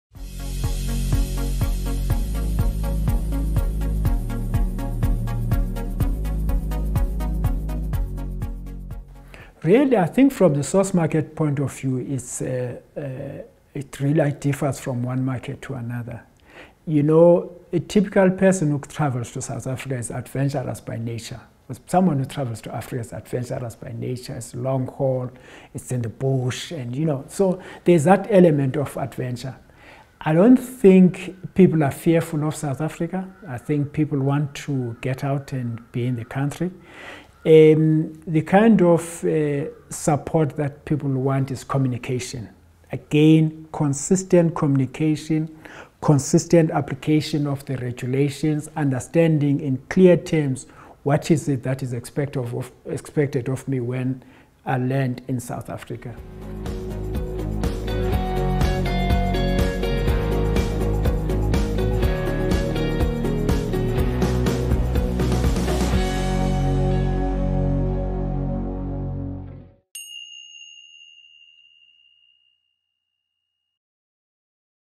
0, Blacky Komani How do you instill trust in the industry
In this video, TBCSA Chairman Blacky Komani describes how the tourism industry will need to instil trust that the country is safe to visit.